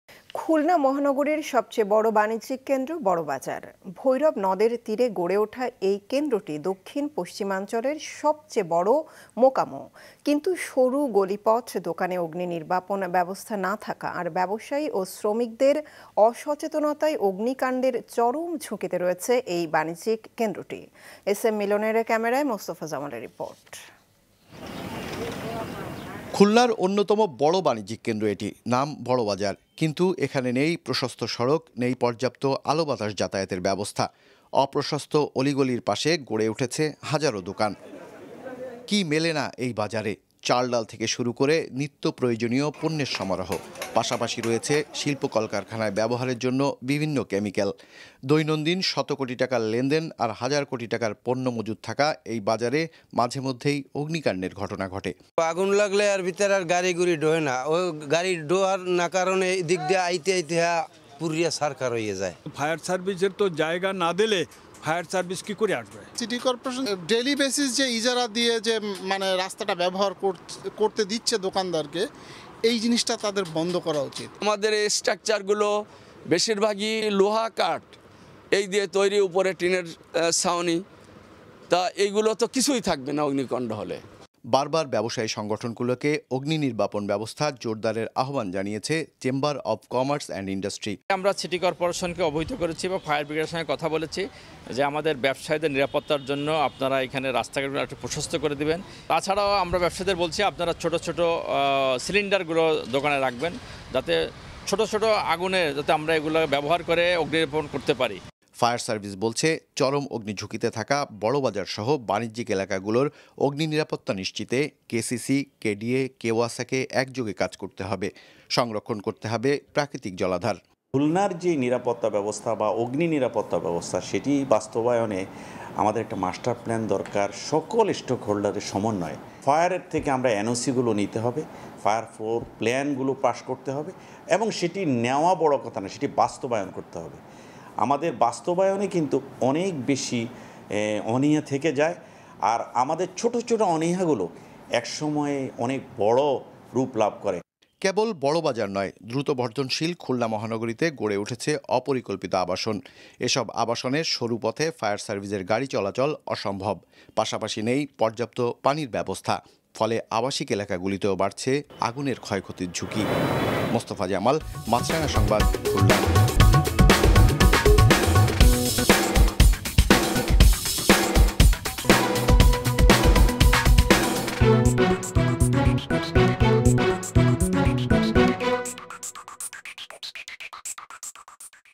ঝুঁকিতে খুলনার বড় বাজার । Maasranga News
খুলনা মহানগরীর সবচেয়ে বড় বাণিজ্যিক কেন্দ্র বড় বাজার। ভৈরব নদের তীরে গড়ে উঠা এই কেন্দ্রটি দক্ষিণ-পশ্চিমাঞ্চলের সবচেয়ে বড় মোকামও। কিন্তু সরু গলিপথ, দোকানে অগ্নি নির্বাপণ ব্যবস্থা না থাকা, আর ব্যবসায়ী ও শ্রমিকদের অসচেতনায় অগ্নিকাণ্ডের চরম ঝুঁকিতে রয়েছে এই বাণিজ্যিক কেন্দ্রটি।

2 Bir Uttam Ziaur Rahman Road (New Airport Road)